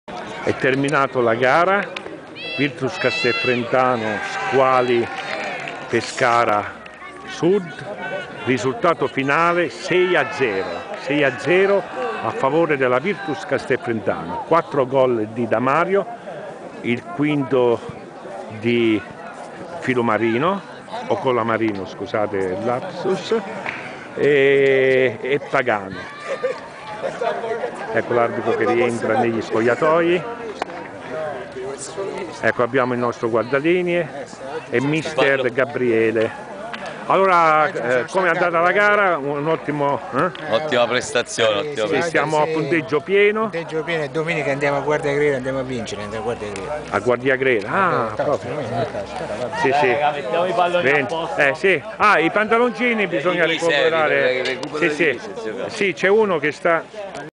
VIRTUS CASTEL FRENTANO SECONDA CATEGORIA GIRONE E. Stag. 2017 - 2018
Dopo il quarto gol di PAGANO si è chiuso il secondo tempo a nostro vantaggio per 4 - 0 . Nel secondo Tempo non c'è storia . Altri due gol rifilati all'avversario di turno Squali Pescara Sud.
SIAMO PRIMI !|